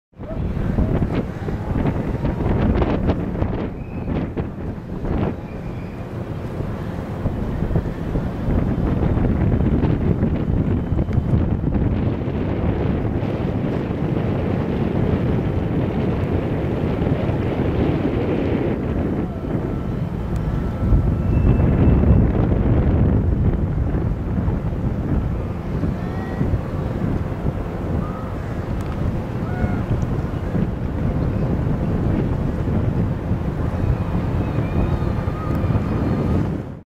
Port of Vancouver Panorama
Standing on the pier in Crab Park a short panorama shot during a gorgeous summer day in Vancouver